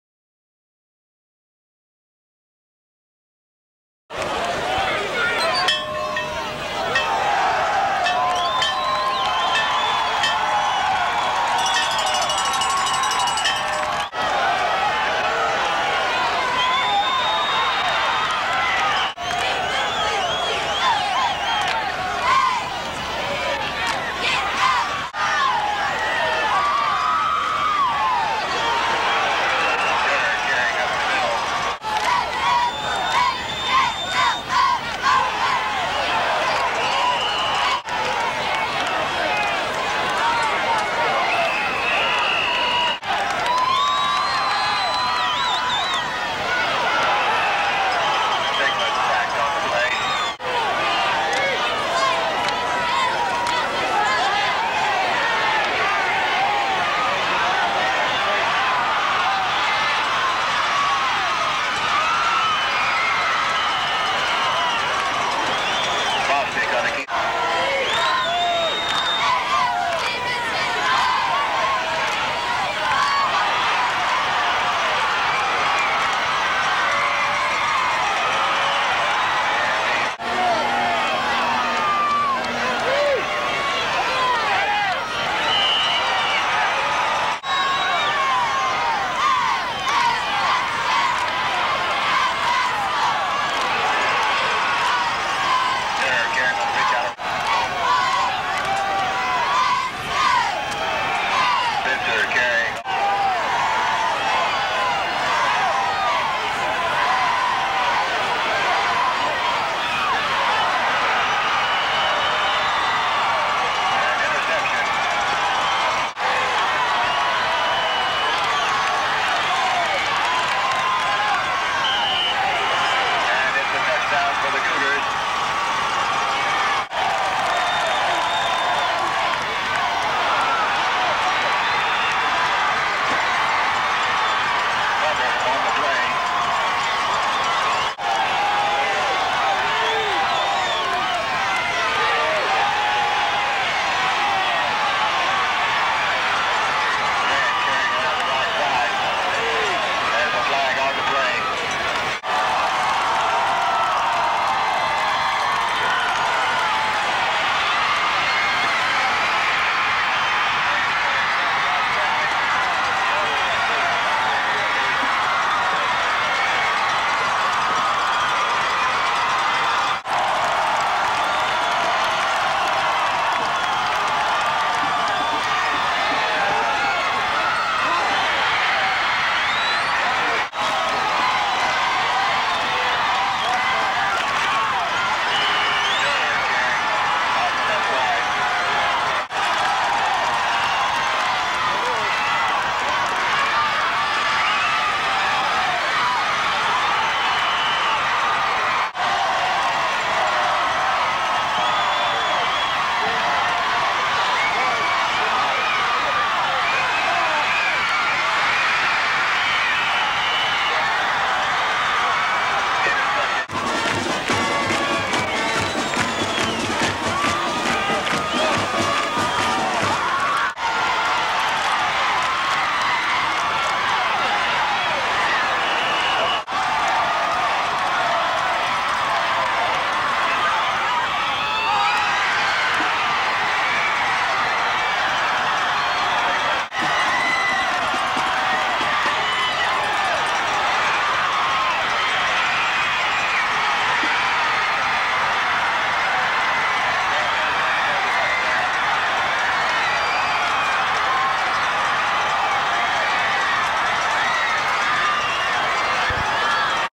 1985 2A Playoffs Rd. 2: Charlotte Catholic 28 New. Conover 22
From 1985  the game known as THE COMEBACK! Catholic found themselves down 22-0 in the middle of the 2nd quarter. A TD pass from Mark Bostic to Steven Schachner before the half made it 22-8. Catholic would shutout the Red Devils in the 2nd half tying the game late in the 4th quarter at 22 all. A stop by CC in the 1st overtime gave the cougars the ball and they score on the 1st play of OT winning it 28-22!!